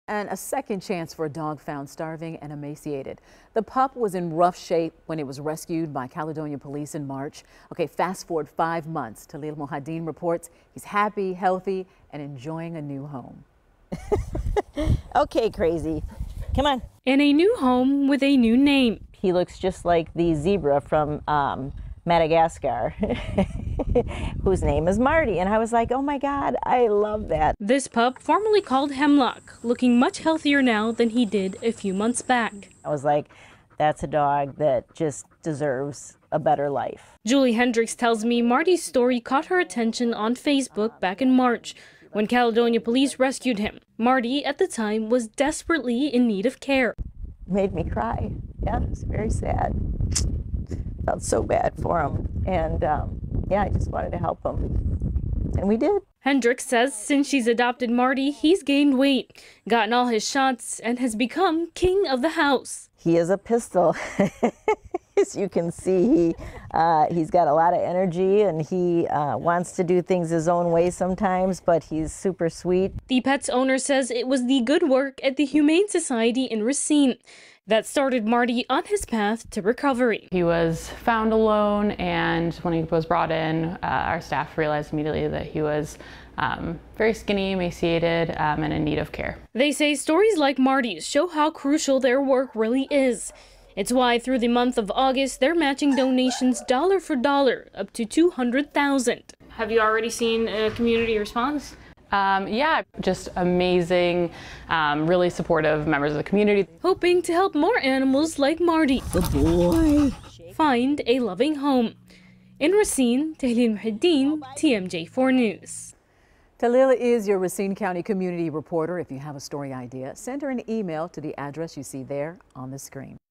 Woman adopts, helps rehabilitate dog found emaciated
After being found emaciated by Caledonia police in March, a pup formerly known as Hemlock has been adopted and is looking much healthier.

The dog’s new owner Julie Hendricks told TMJ4’s Tahleel Mohieldin that a Facebook post about the rescue’s condition caught her attention.

“It made me cry,” she said. “I was like that’s a dog that deserves a better life.”

RELATED: Hemlock's second chance: Emaciated stray dog rescued by Caledonia police

After watching and waiting for him to become available for adoption, Hendricks was able to give the dog a new home along with a new name.